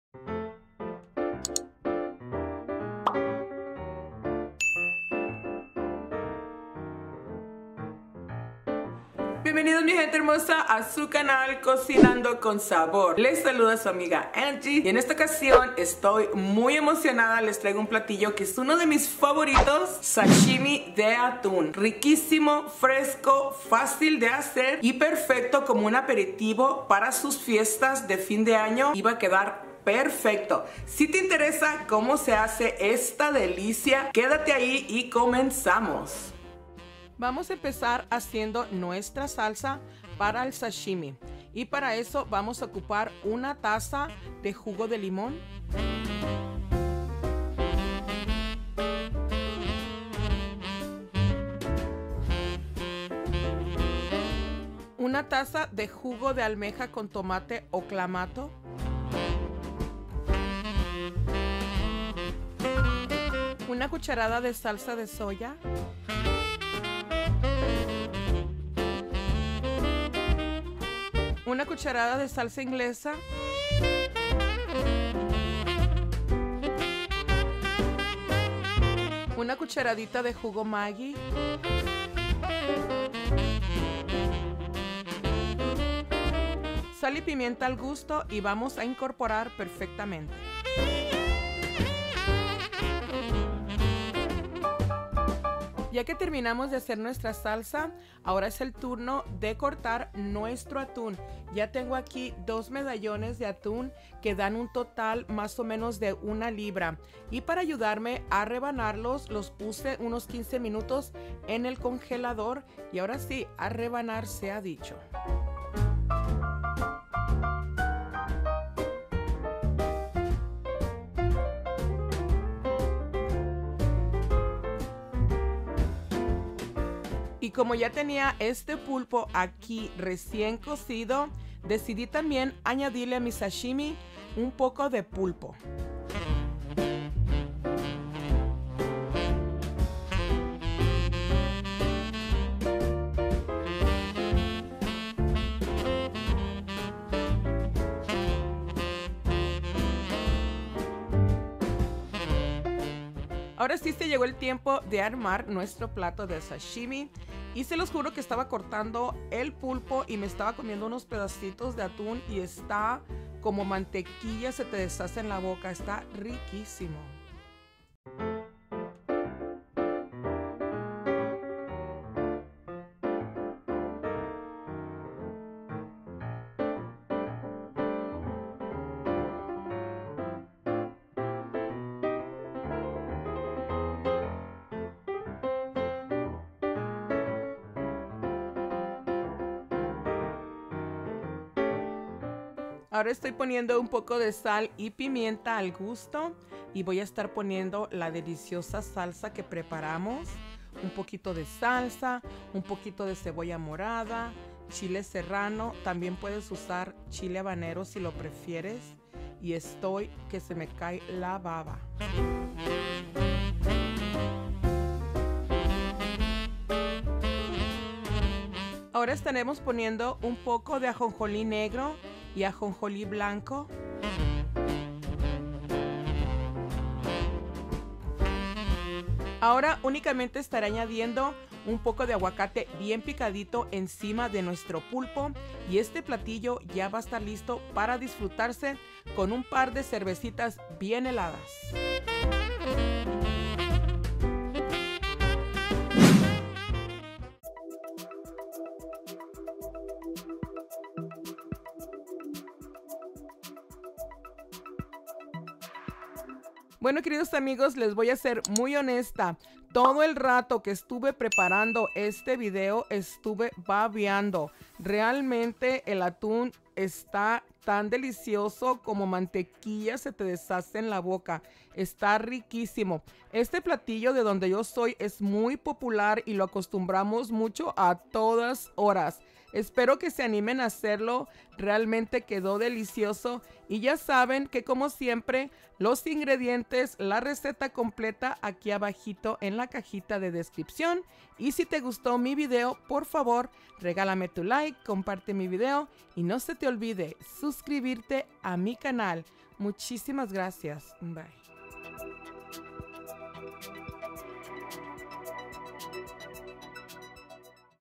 SASHIMI DE ATUN Y PULPO
🐟1lb de atun
🐙1/2 lb de pulpo cocido
🐟1/2 cebolla morada en julianas
🐟2chiles serranos
🐟sal y pimienta al gusto
🐟aguacate en cuadritos al gusto
🐟1tsp de ajonjolí negro
🐟1tsp de ajonjolí blanco

✅Para la salsa petrolera
💚1taza de jugo de limón verde
💚1taza de jugo de almeja con tomate ( Clamato)
💚1tbs de salsa de soya
💚1tbs de salsa inglesa
💚1tsp de jugo magi

💖Instagram Cocinando _Con_Sabor